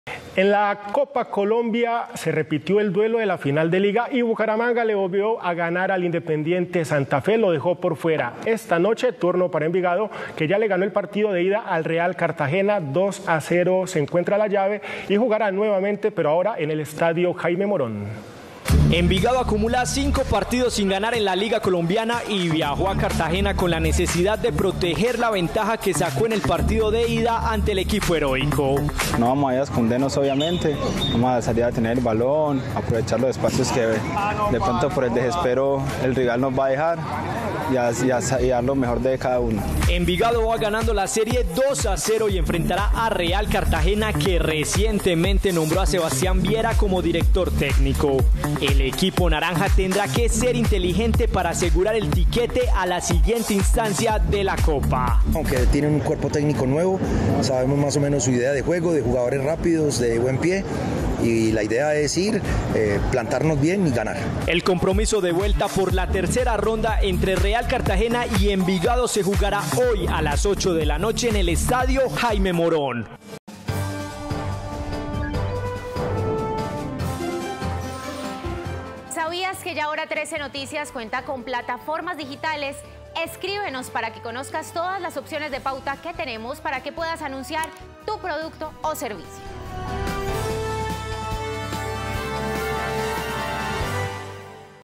Envigado visita esta noche al Real Cartagena por la tercera ronda de la Copa Colombia
El equipo naranja ganò el partido de ida 2 goles por cero